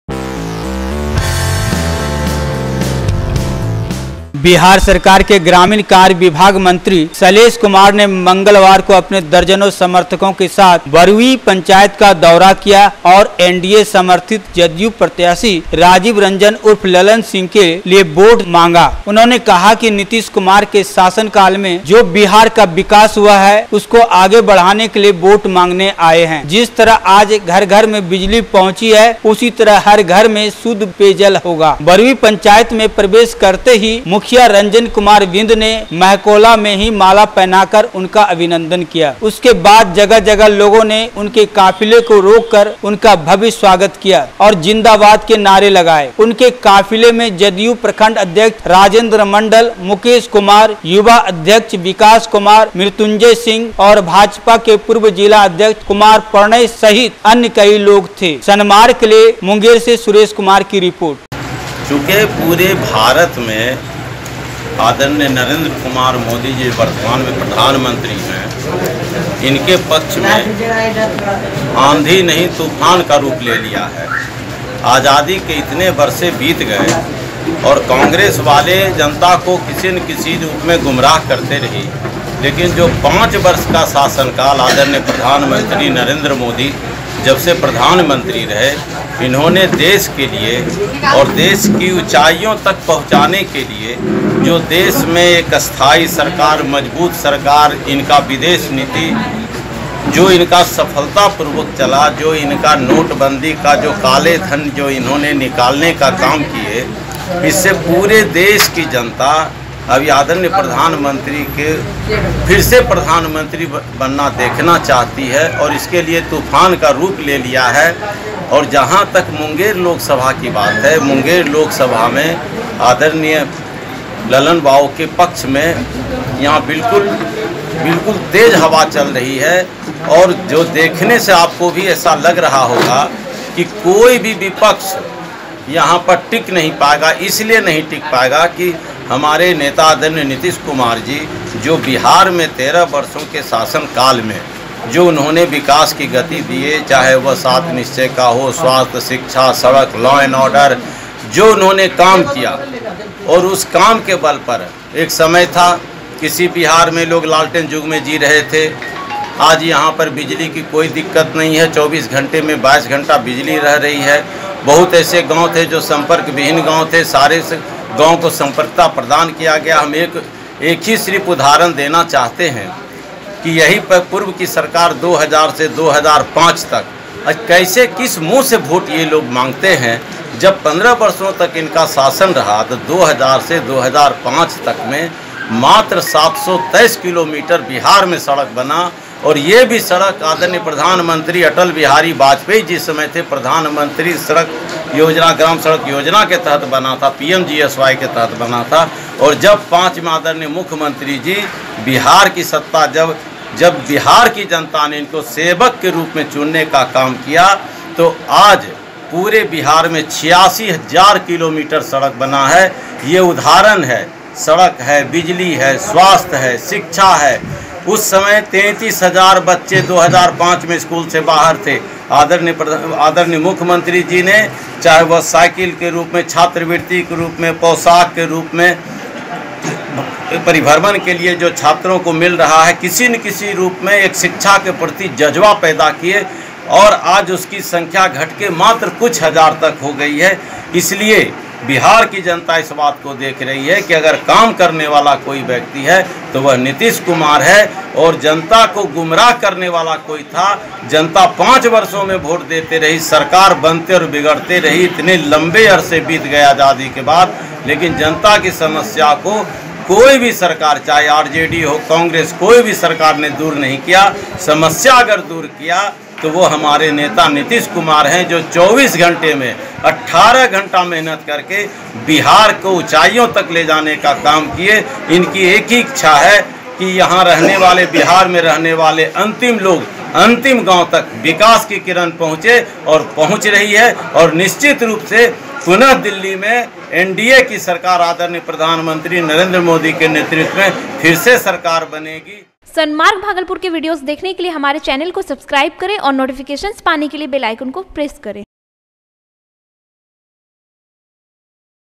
ग्रामीण कार्य विभाग मंत्री शैलेश कुमार ने किया बरुई पंचायत का दौरा
मुंगेर : बिहार सरकार के ग्रामीण कार्य विभाग मंत्री शैलेश कुमार ने मंगलवार को अपने दर्जनों समर्थकों के साथ बरुई पंचायत का दौरा किया और एनडीए समर्थित जदयू प्रत्याशी राजीव रंजन उर्फ ललन सिंह के लिए वोट मांगा। उन्होंने  कहा कि नीतीश कुमार के शासनकाल में जो बिहार का विकास हुआ है, को आगे बढ़ाने के लिए वोट मांगने आए हैं। जिस तरह आज घर घर में बिजली पहुंची है, उसी तरह हर घर में शुद्ध पेयजल होगा। बरुई पंचायत में प्रवेश करते ही मुखिया रंजन कुमार बिंद ने महकोला में ही माला पहनाकर उनका अभिनंदन किया। उसके बाद जगह-जगह लोगों ने उनके काफिला को रोककर उनका भव्य स्वागत किया और जिंदाबाद के नारे लगाए। उनके  काफिले में जदयू प्रखंड अध्यक्ष राजेंद्र मंडल, मुकेश कुमार, युवा अध्यक्ष विकास कुमार, मृत्युंजय सिंह, भाजपा के पूर्व जिला अध्यक्ष कुमार प्रणय सहित अन्य कई लोग  थे।